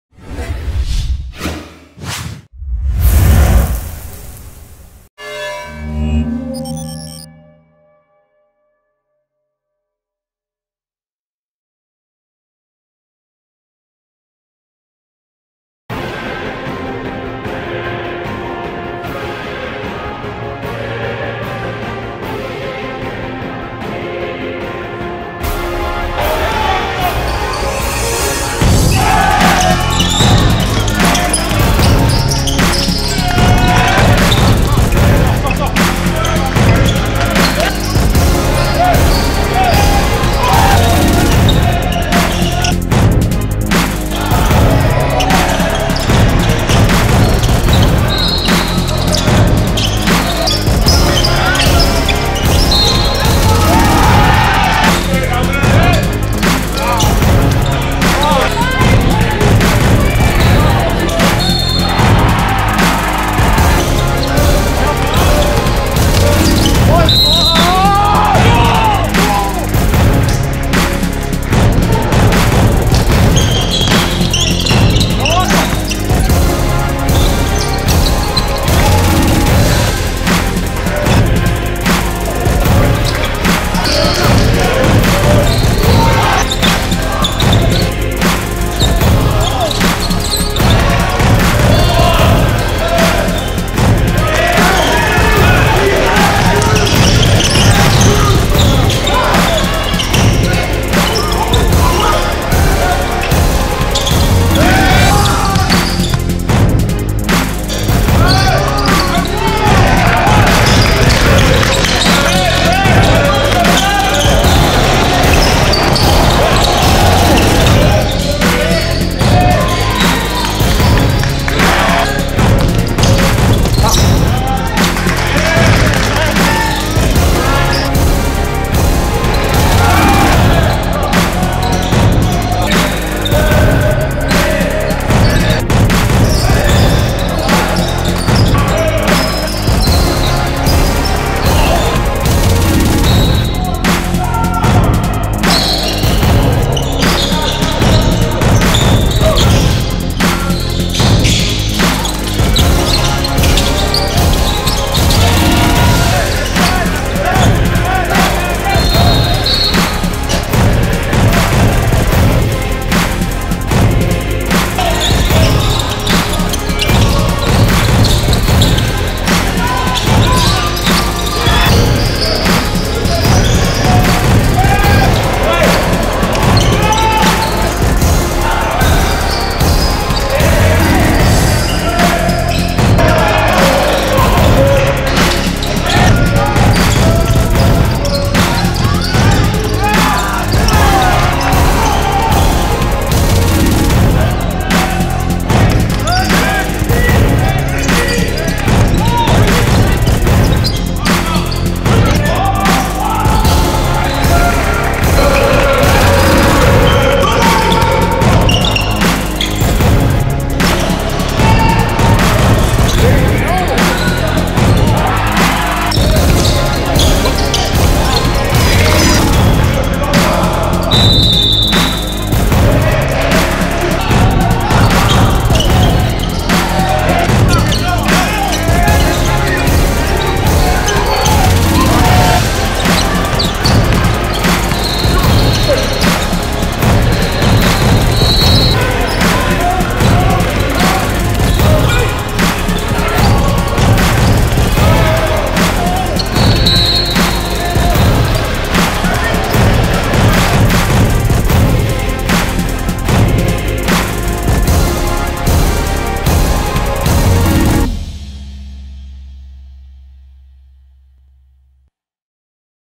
Guillaume Pepin 6'6 SG/SF Class of 2018 | Official Half Season Mixtape
Guillaume Pepin is a shooting guard/ Small forward of the class of 2018. He is very versatile and is a matchup nightmare. He’s too quick for bigs and if you put a guard on him he will use his size and post skills to score in the paint. In his third year in Brebeuf, he has added improved ball handling and passing, which has made him one of the top players in league.

Player profile:
Position: Shooting Guard/Small Forward
Height: 6'6
Hometown: Montreal, QC
Class: 2018
Current Team: College Jean-de-Brebeuf
Past team: Brookwood elite / College Jean-de-Brébeuf / Sphere Basketball / TEAM QC